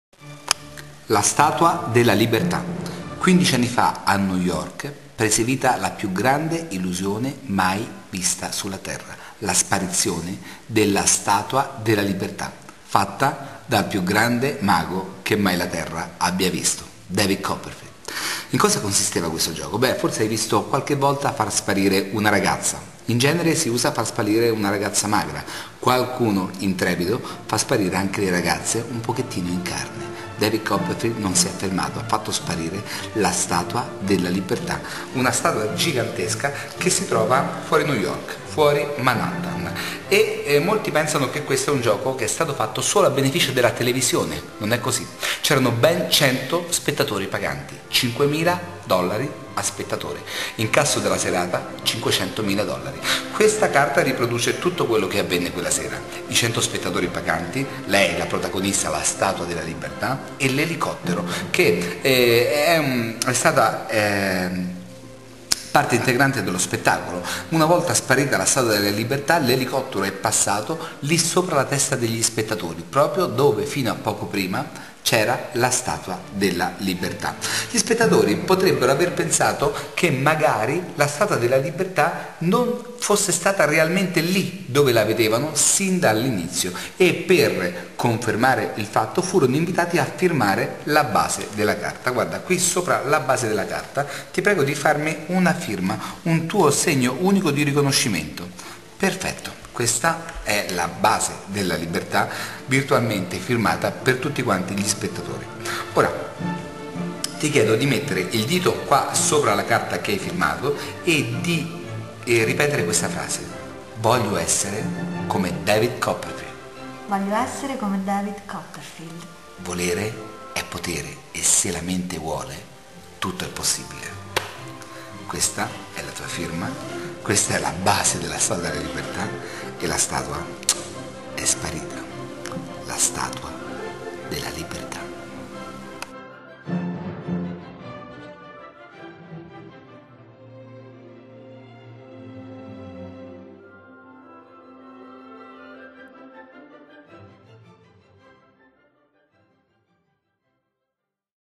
Sparizione della statua della libertà,-- 50 commenti in 24 ore-- grazie.
David Copperfiled e la sparizione della statua della libertà Salottino. ... Il tutto si svolge sull`isola dove sta la statua della liberta` ...giochi di prestigio